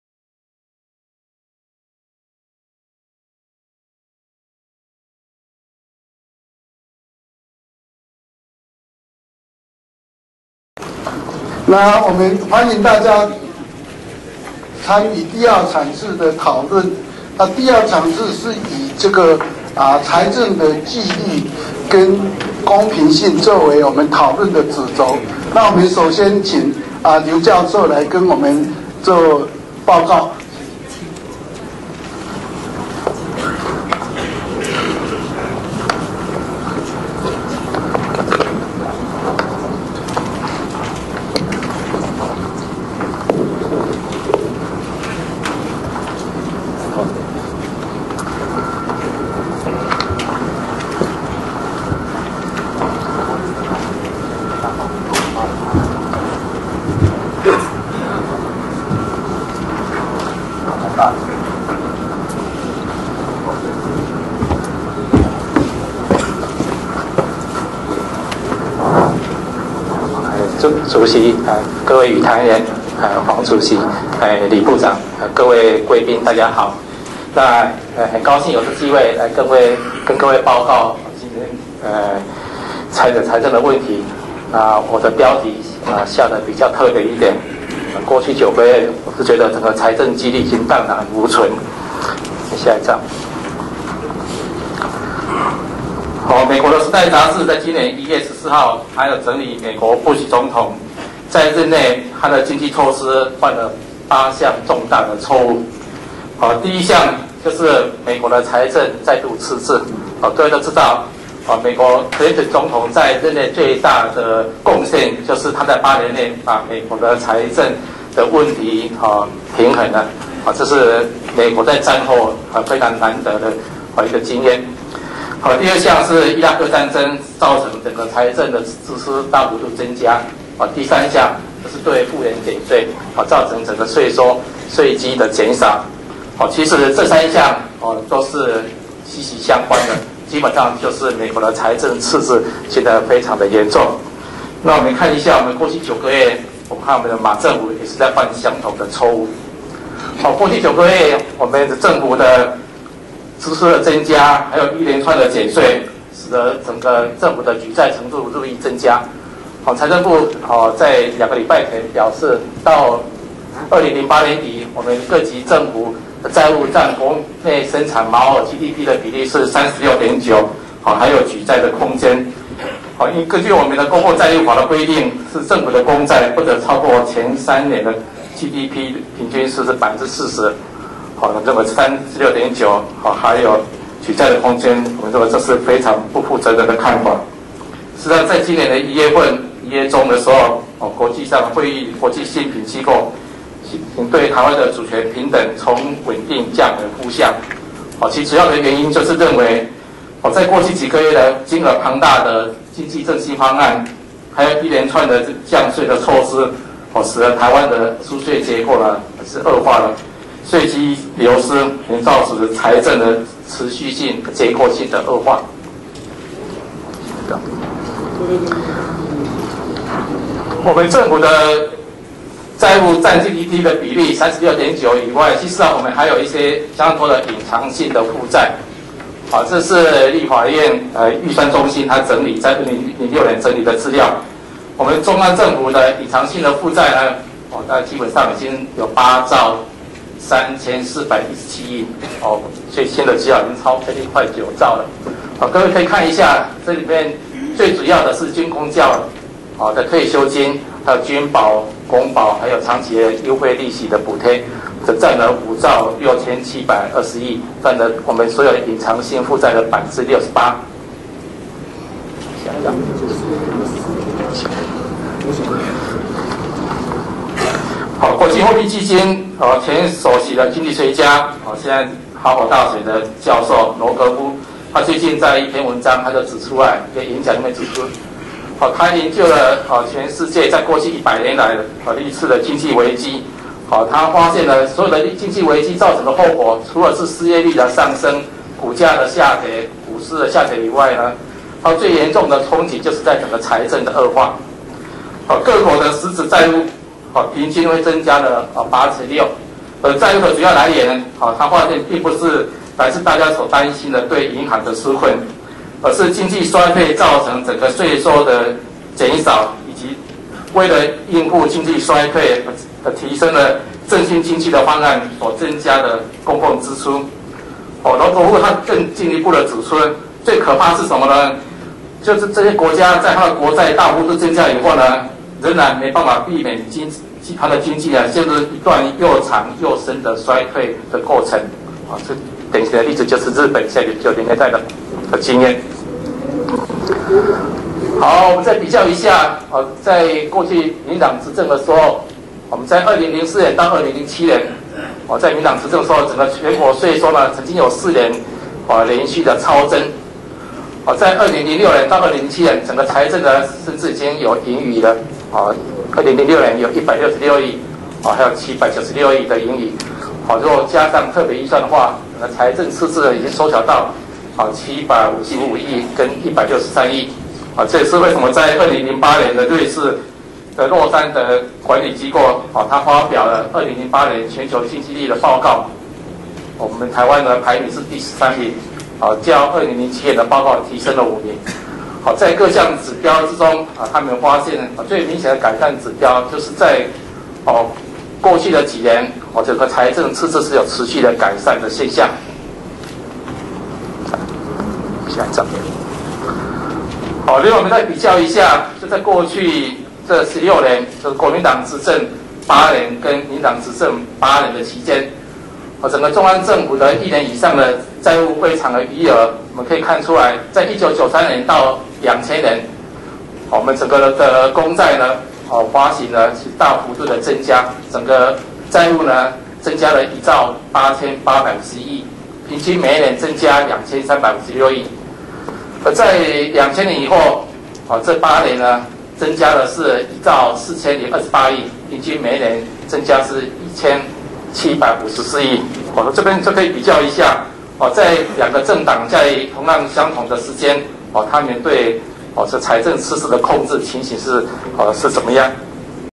民間國是會議第二場-劉景添111
團結民間在野的力量，制衡國民黨一黨獨大的現象，民主進步黨與台灣團結聯盟共同發起２００９台灣民間國是會議